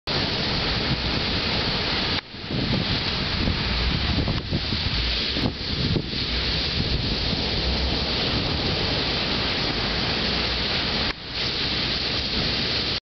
Gorgeous Wave
I know... sounds silly.  Just watch!  The wave is perfect.  About an 8 or 9 foot wall, too.